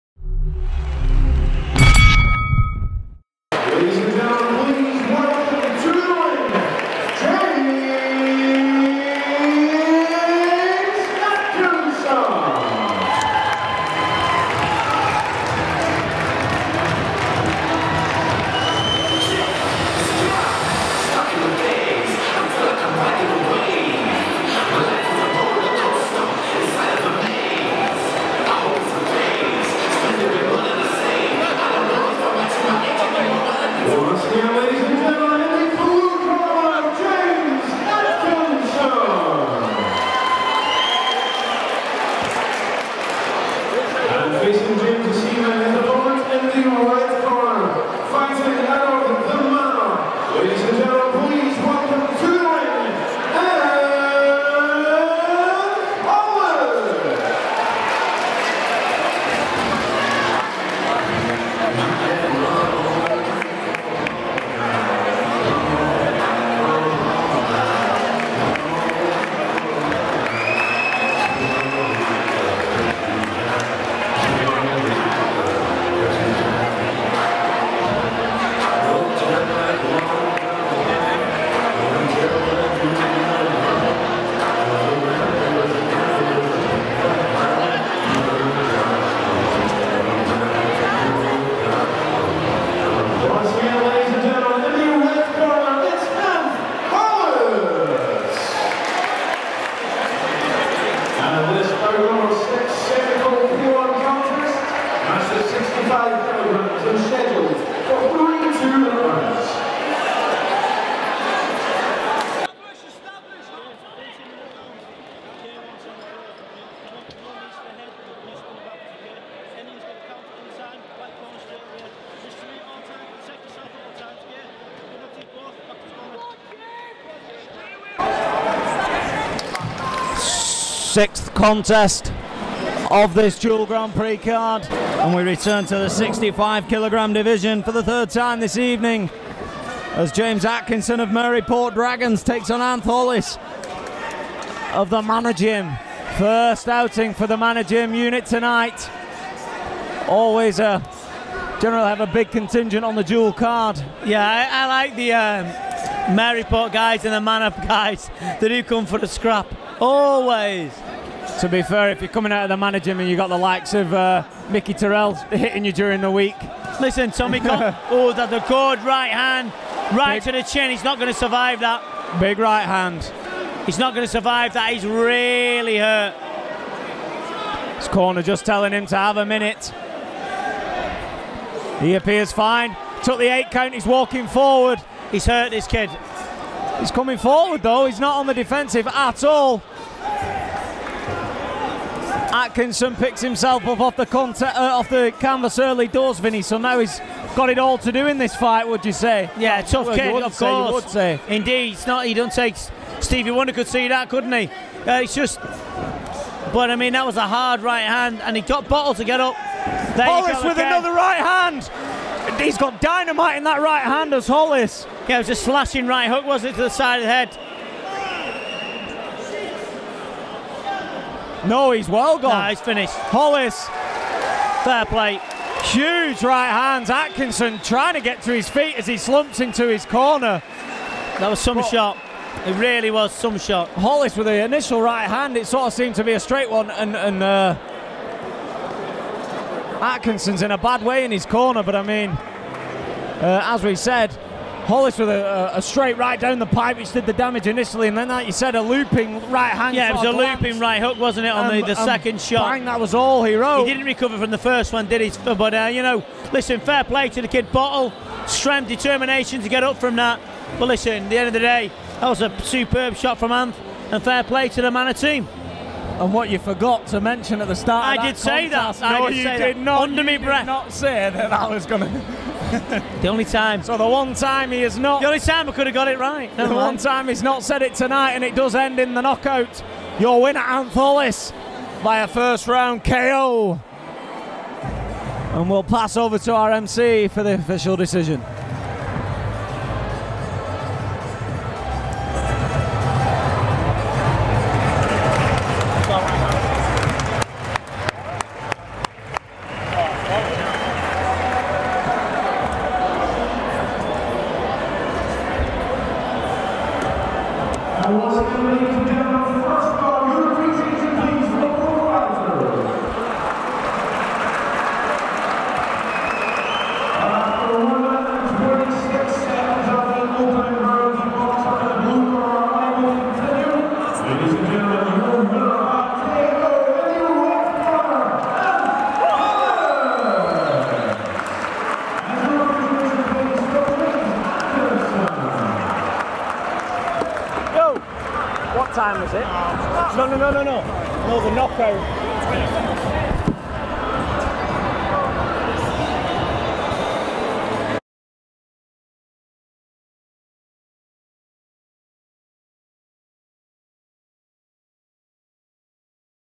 DUEL GRAND PRIX / KICKBOXING / ANTH HOLLIS V JAMES ATKINSON / PEEP MAGAZINE
DUEL GRAND PRIX ANTH HOLLIS ( The Manor Gym ) \/ JAMES ATKINSON ( Maryport Dragons ) COMMENTARY BY CHRIS CLARKE & VINNY SHOREMAN. FILMED @ WALKER ACTIVITY DOME NEWCASTLE / PEEP MAGAZINE PRO COMBAT SPORTS.